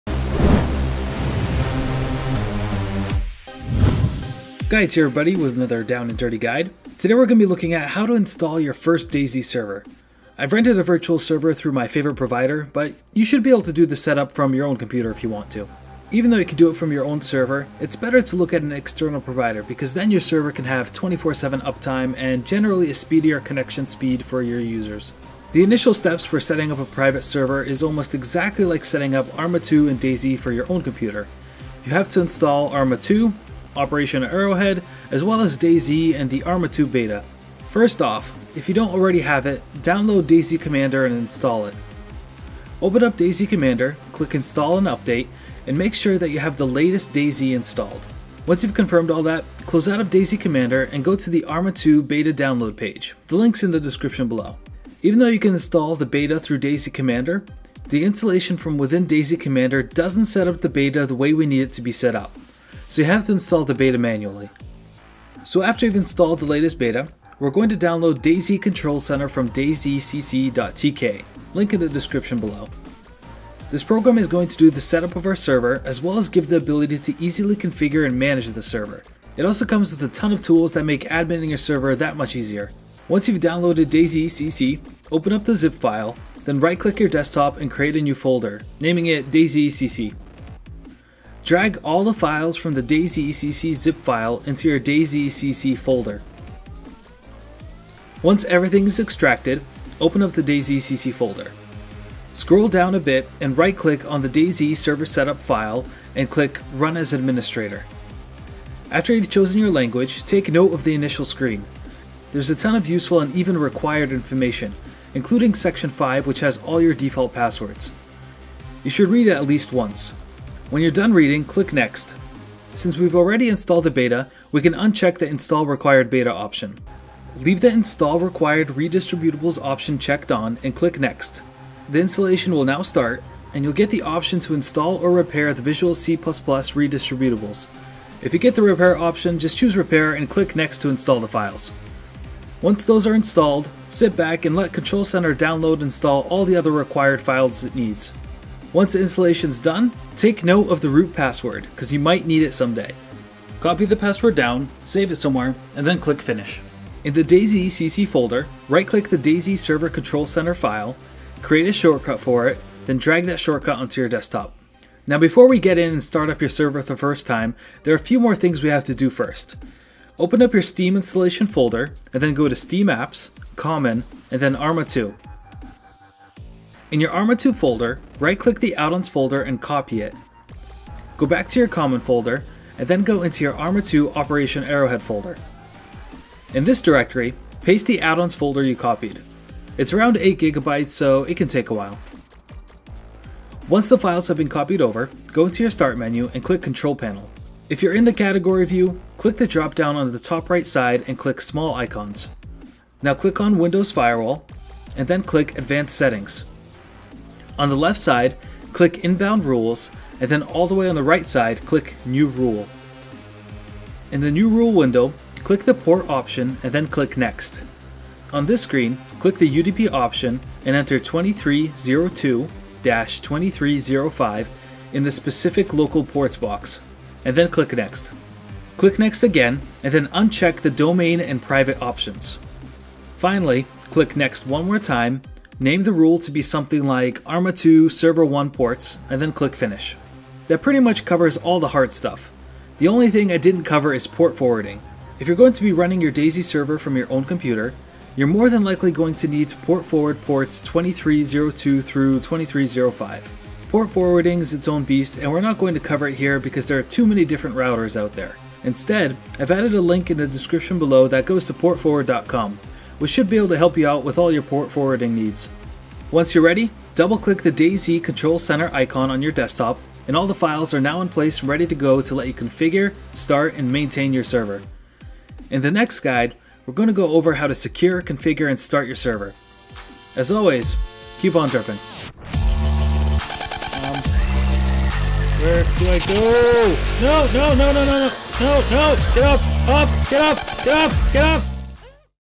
DayZ MOD Server Series - Episode 1 - Installation
FOR THOSE HAVING INSTALL/Database/MySQL ISSUES:
Try this out:
1. Delete you @dayzcc and @dayzcc_config folder, and keep your ARMA2OA folder open.
2. Go and start the setup of Control Center again, and as it's installing watch your ARMA2OA folder.
3. Once the @dayzcc folder appears, open it, then open the MYSQL folder, and finally the BIN folder.
4. Once the control center installer gets to the "Starting MySQL" part of the setup, quickly double-click the MYSQLD.exe file in your BIN folder. The databases should install correctly this time,